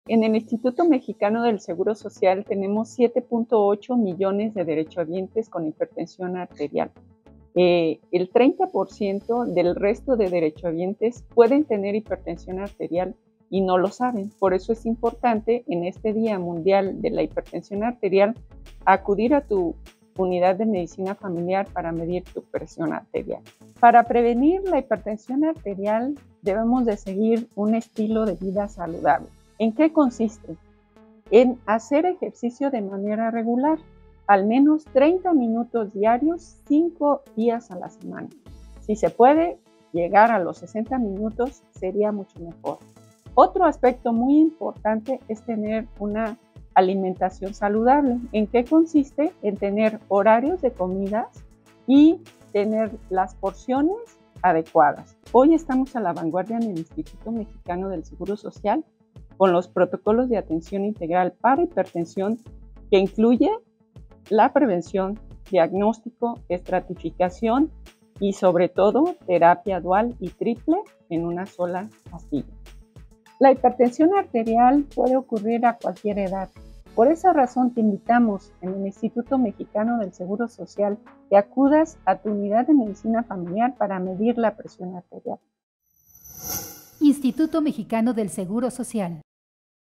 Día Mundial de la HipertensiónArterial.
🩺 La HipertensiónArterial es una enfermedad que no presenta síntomas, pero el sobrepeso, obesidad y el sedentarismo aumentan el riesgo de padecerla.